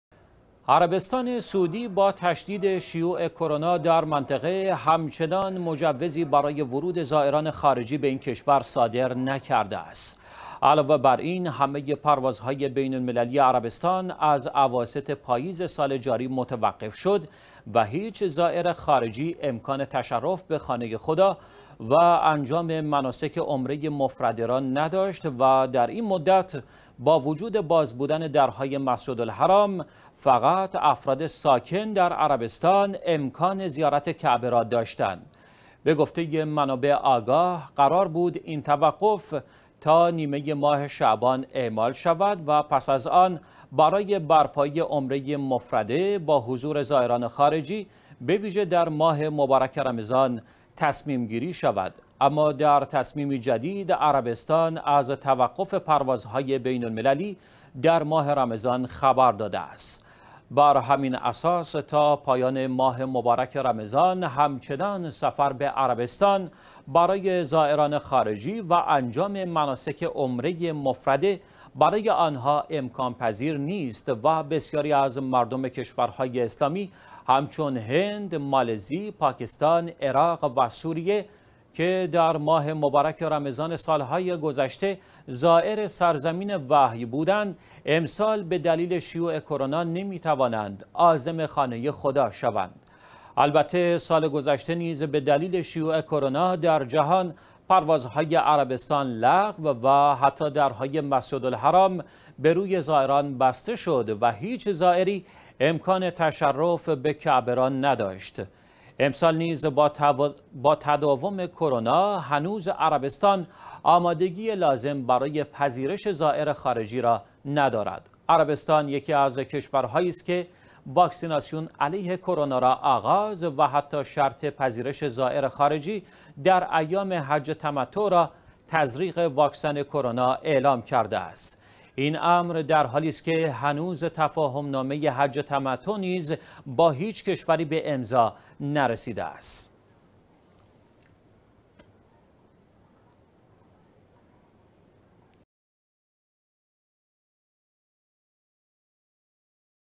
عربستان تا پایان ماه رمضان زائر خارجی نمی‌پذیرد
عربستان سعودی با تشدید شیوع کرونا در منطقه همچنان مجوزی برای ورود زائران خارجی به این کشور صادر نکرده است.
علاوه بر این، همه پروازهای بین‌المللی عربستان از اواسط پاییز سال جاری متوقف شد و هیچ زائر خارجی امکان تشرف به خانه خدا و انجام مناسک عمره مفرده را نداشت و در این مدت با وجود باز بودن درهای مسجدالحرام، فقط افراد ساکن در عربستان امکان زیارت کعبه را داشتند.
به گفته منابع آگاه قرار بود این توقف تا نیمه ماه شعبان اعمال شود و پس از آن برای برپایی عمره مفرده با حضور زائران خارجی به‌ویژه در ماه مبارک رمضان تصمیم‌گیری شود. اما در تصمیمی جدید، عربستان از توقف پروازهای بین‌المللی در ماه رمضان خبر داده است.
بر همین اساس، تا پایان ماه مبارک رمضان همچنان سفر به عربستان برای زائران خارجی و انجام مناسک عمره مفرده برای آن‌ها امکان‌پذیر نیست و بسیاری از مردم کشورهای اسلامی همچون هند، مالزی، پاکستان، عراق و سوریه که در ماه مبارکه رمضان سال‌های گذشته زائر سرزمین وحی بودند، امسال به دلیل شیوع کرونا نمی‌توانند عازم خانه خدا شوند.
البته سال گذشته نیز به دلیل شیوع کرونا در جهان، پروازهای عربستان لغو و حتی درهای مسجدالحرام به روی زائران بسته شد و هیچ زائری امکان تشرف به کعبه را نداشت. امسال نیز با تداوم کرونا هنوز عربستان آمادگی لازم برای پذیرش زائر خارجی را ندارد.
عربستان یکی از کشورهایی است که واکسیناسیون علیه کرونا را آغاز و حتی شرط پذیرش زائر خارجی در ایام حج تمتع را تزریق واکسن کرونا اعلام کرده است.
این امر در حالی است که هنوز تفاهم‌نامه حج تمتع نیز با هیچ کشوری به امضا نرسیده است.